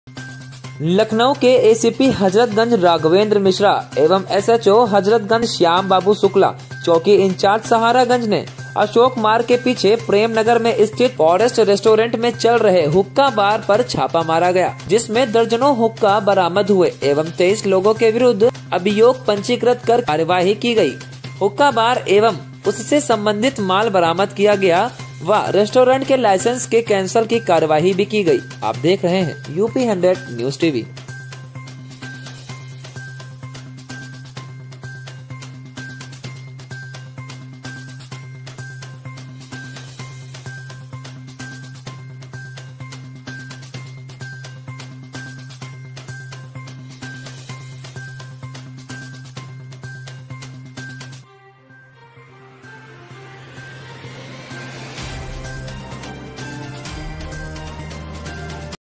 Lucknow सहारागंज के फॉरेस्ट रेस्टोरेंट के हुक्का बार में पड़ा छापा
लखनऊ: एसीपी हजरतगंज राघवेंद्र मिश्रा एवं sho हजरतगंज श्याम बाबू शुक्ला चौकी इंचार्ज सहारागंज ने अशोक मार्ग के पीछे प्रेम नगर में स्थित फॉरेस्ट रेस्टोरेंट में चल रहे हुक्का बार परछापा मारा गया।

जिसमें दर्जनों हुक्का बरामद हुआ एवं 23 लोगों के विरुद्ध अभियोग पंजीकृत कर के कार्यवाही प्रचलित है। हुक्का बार एवं उससे संबंधित माल बरामद किया गया है रेस्टोरेंट के लाइसेंस के कैंसिलेशन की कार्रवाई अलग से प्रचलित है।

हमारे चैनल से जुड़ने के लिए हमें व्हाट्सएप करें